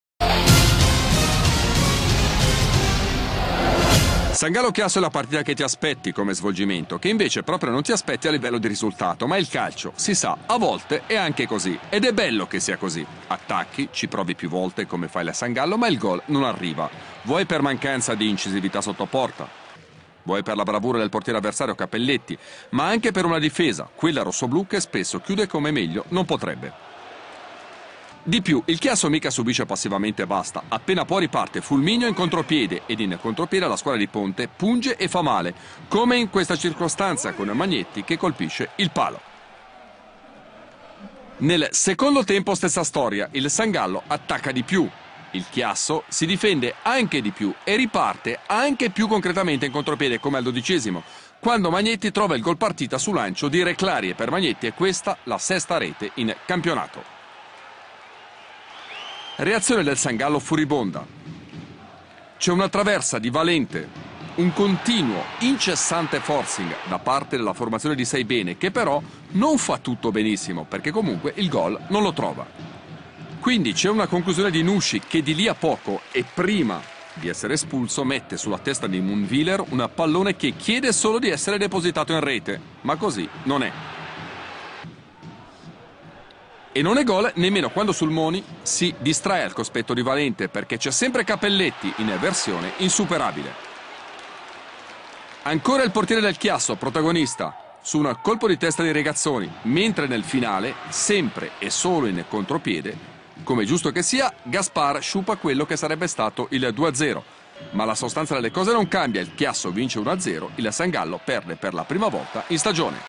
FC St. Gallen - FC Chiasso (Saison 2011/2012)
Der aktuelle Leader empfängt zu Hause den FC Chiasso.